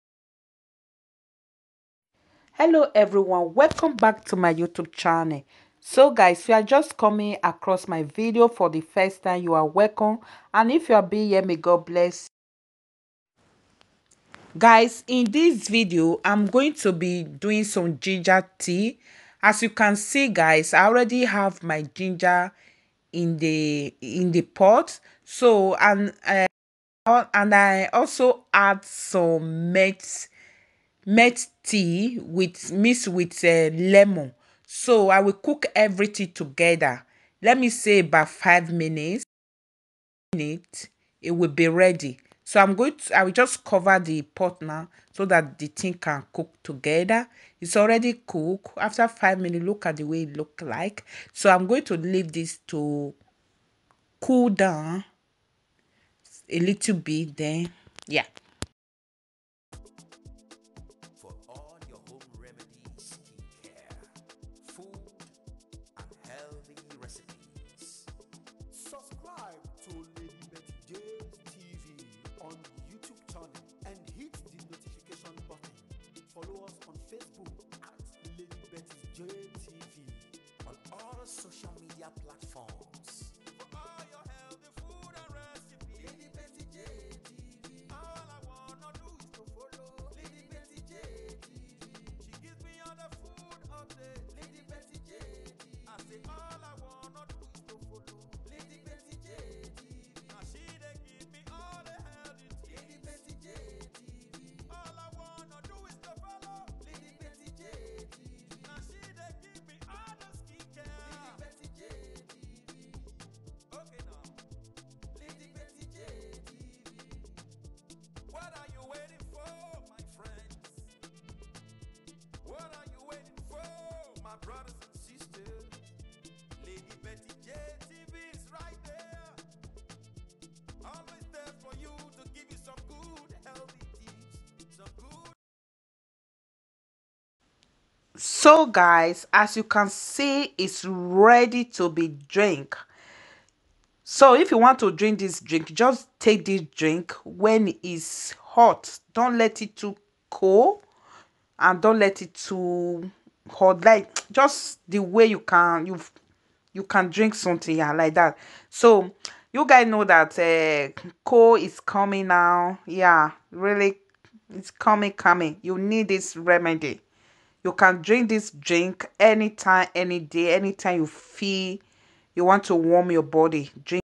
Drink this to get cold out of your body
ingredients
water
ginger
mine tea
lemon
Cook together ❤️